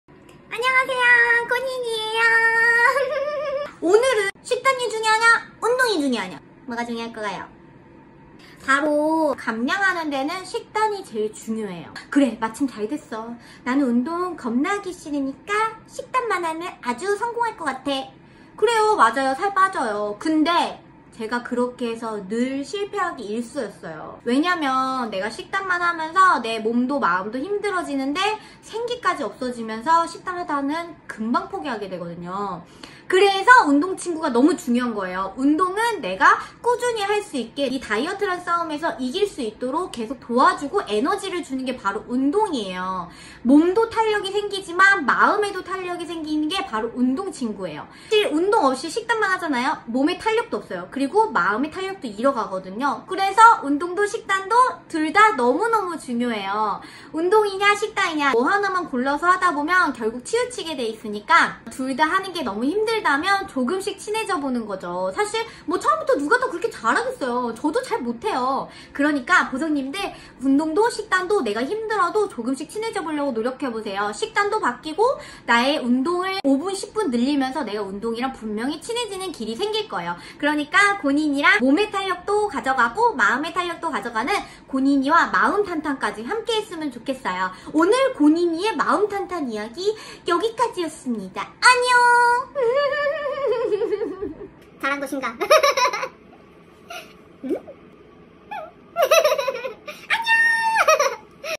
살빼기? 운동&식단 뭐가더 중요할까?😈❤️
뭐가 중요할까요?
뭐하나만 과연 중요할까요???
다이어트 감량엔 식단이 답이지만,
그 장기간 나와의 싸움을 가능하게
만들어주는게 운동이라면?☺️❤️
둘다중요한 거겠죠?
우리 같이해요
고니니의마음탄탄